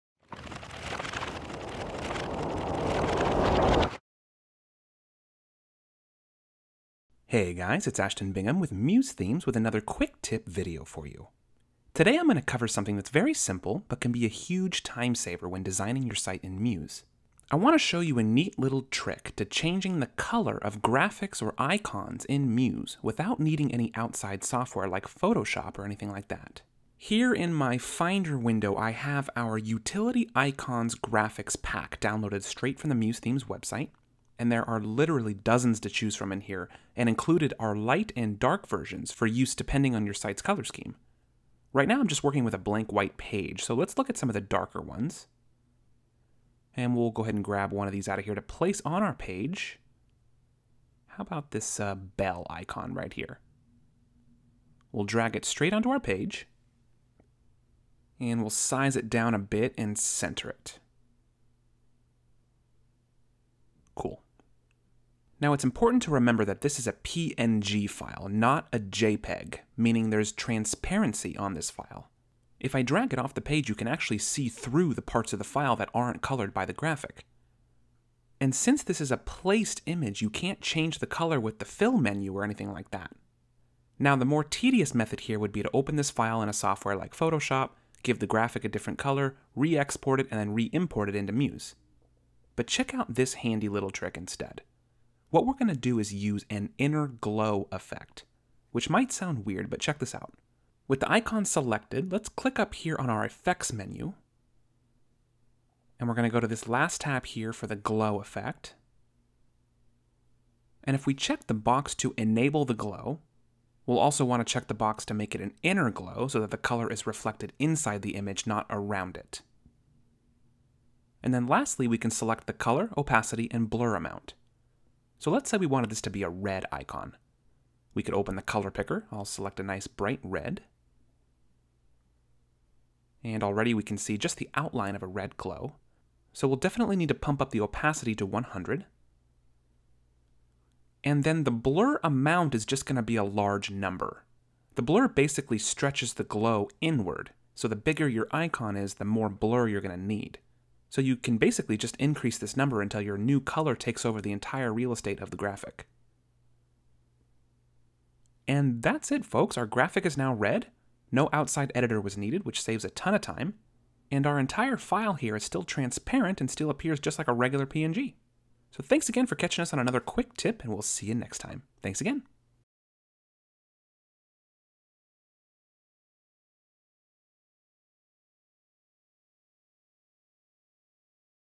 Changing Icon Colors In Muse | Quick Tip by MuseThemes.com
Learn how to change the color of .png icons directly in Muse, without the need to edit in Illustrator or Photoshop.